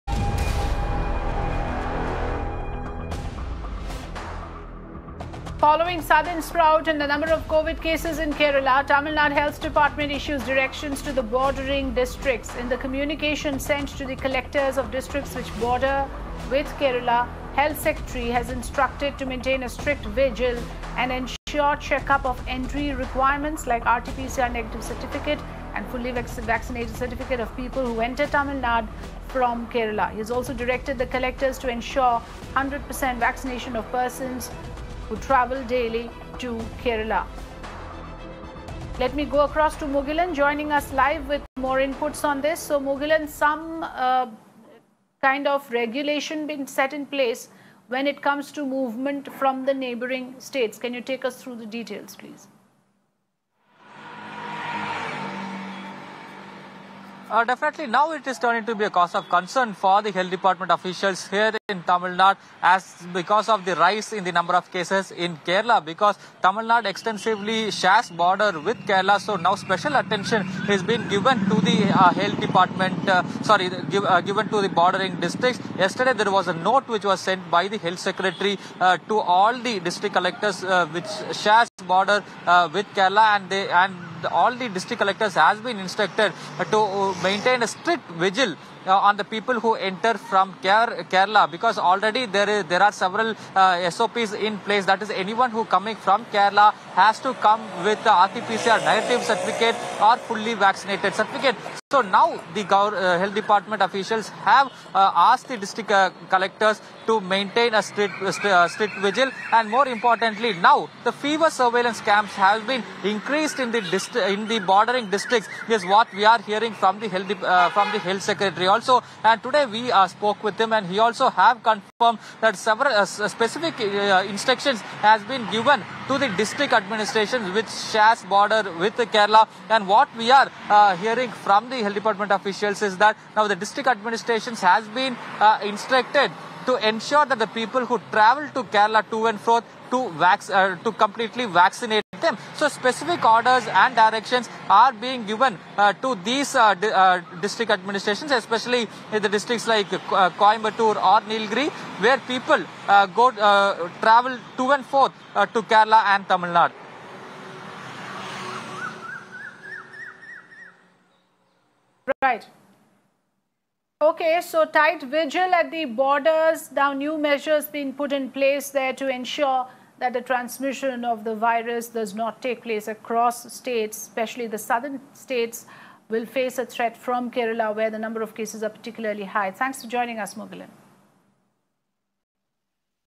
Tamil Nadu sets new Covid norms amidst spike in Kerala
Travelers entering Tamil Nadu from Kerala must either have an RTPCR test report or a certificate stating that they have received both doses of the Covid-19 vaccination, according to the state administration. Individuals should have taken the second dosage at least 14 days before flying, riding, or driving into Tamil Nadu, according to the authorities. The additional regulations were introduced in response to an increase in cases in Kerala. Kerala recorded 31,445 new Covid-19 cases, indicating a high incidence of Covid-19 transmission in the state following Onam. The test positive rate (TPR) has now reached 19%. Watch the video to know more